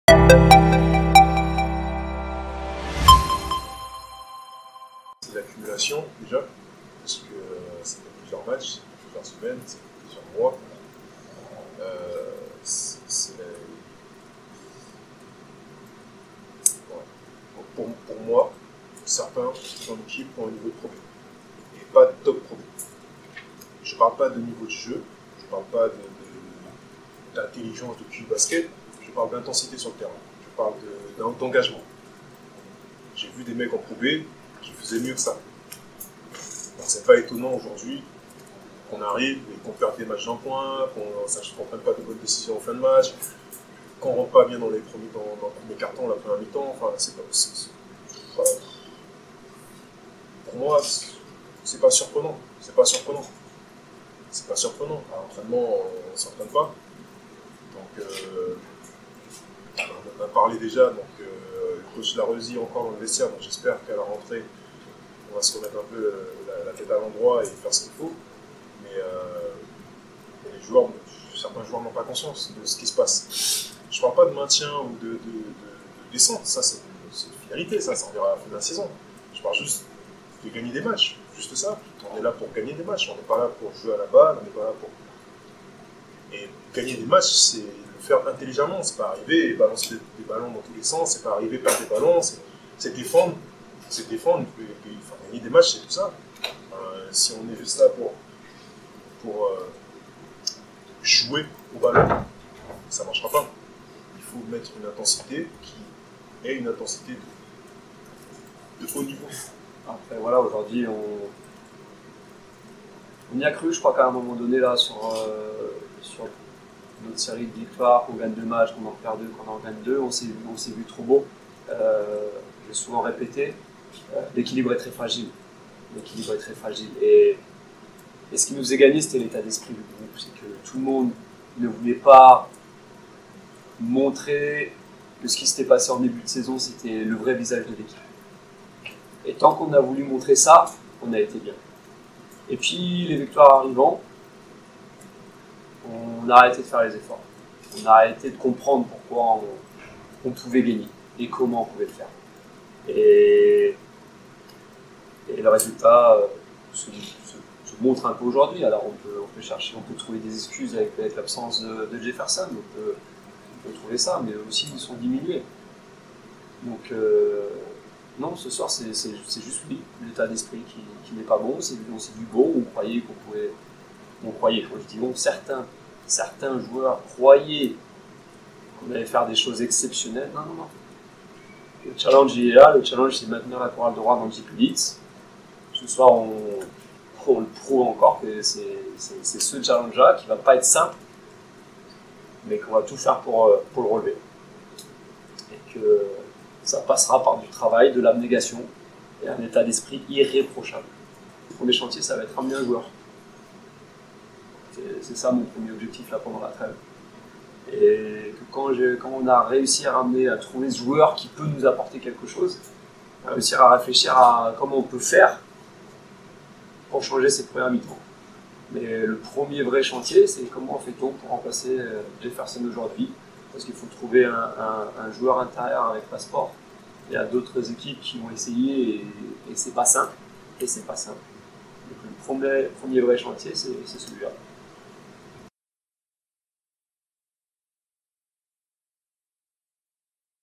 2019/20 Réactions après Chorale - Limoges (73-89, JE J16)
Retrouvez des extraits de la conférence de presse de Steeve Ho You Fat et Maxime Boire après la défaite face à Limoges (73-89), lors de la 16e journée de Jeep® ELITE